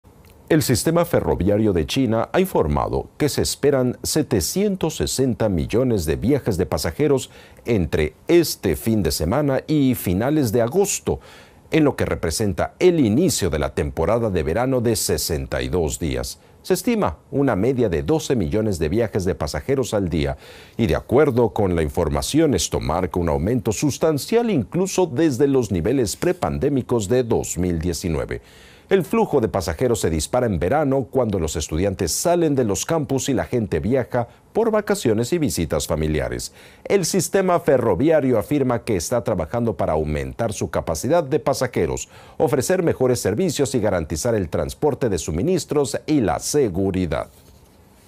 Se esperan 760 millones de viajes de pasajeros durante los 62 días de verano en China
El sistema ferroviario de China ha informado que se esperan 760 millones de viajes de pasajeros entre este fin de semana y finales de agosto, en lo que representa el inicio de la temporada de verano de 62 días. Se estima una media de 12 millones de viajes de pasajeros al día, y de acuerdo con la información, esto marca un aumento sustancial incluso desde los niveles prepandémicos de 2019. El flujo de pasajeros se dispara en verano, cuando los estudiantes salen de los campus y la gente viaja por vacaciones y visitas familiares. El sistema ferroviario afirma que está trabajando para aumentar su capacidad de pasajeros, ofrecer mejores servicios y garantizar el transporte de suministros y la seguridad.